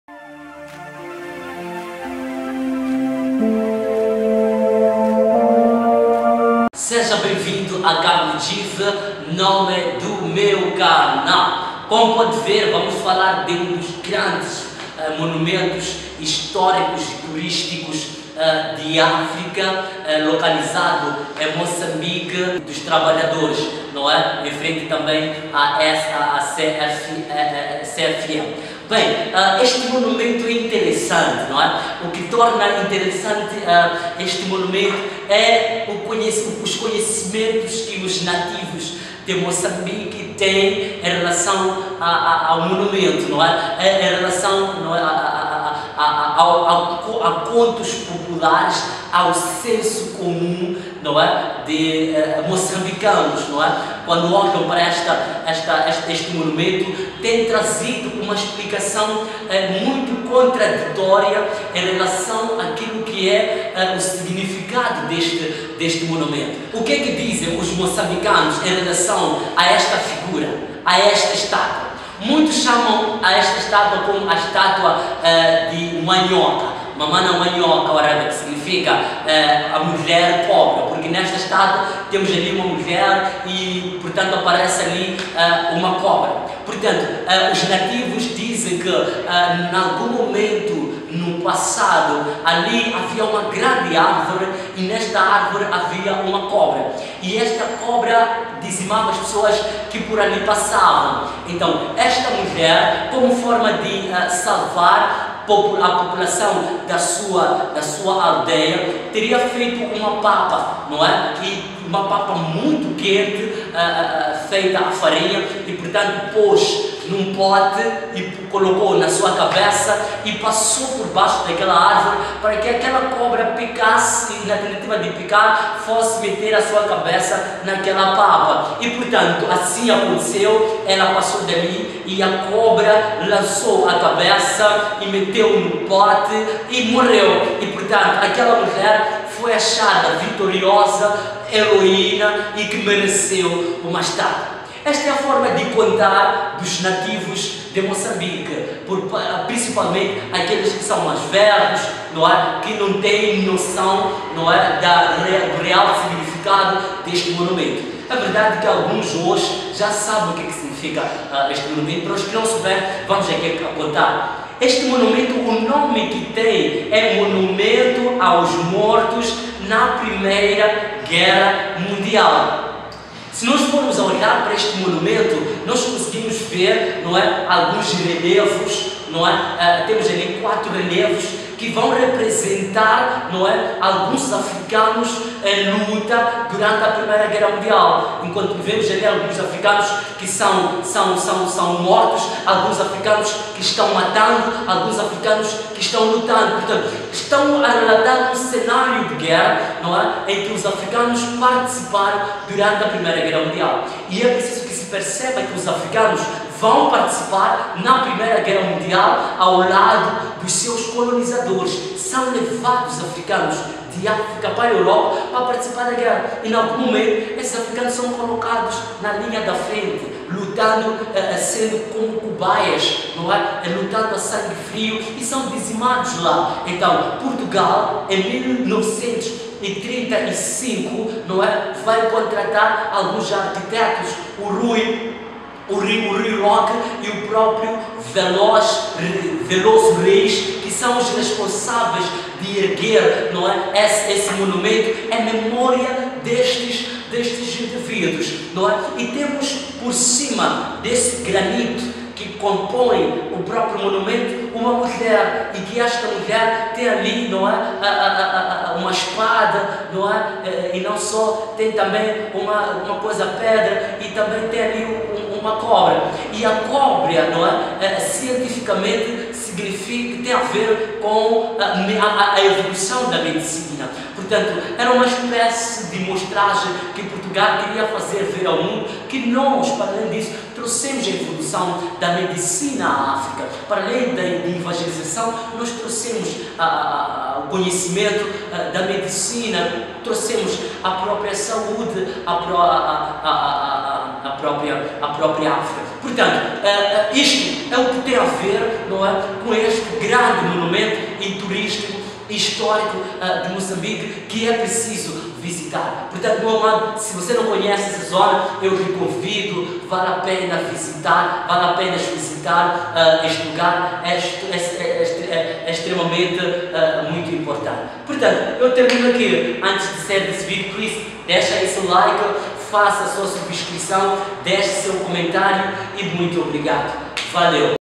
A MÍTICA HISTÓRIA DO MONUMENTO AOS MORTOS DA PRIMEIRA GUERRA MUNDIAL: Africa
Um dos grandes Monumentos históricos e turísticos de Africa, localizado em Moçambique na cidade de Maputo, em frente a praça dos trabalhadores e Caminhos de Ferro de Moçambique.
Esses Monumento, tem suscitado divergência em relação ao seu significado.
Confira neste Vídio as duas versões em relação a seu significado

Muito Obrigado!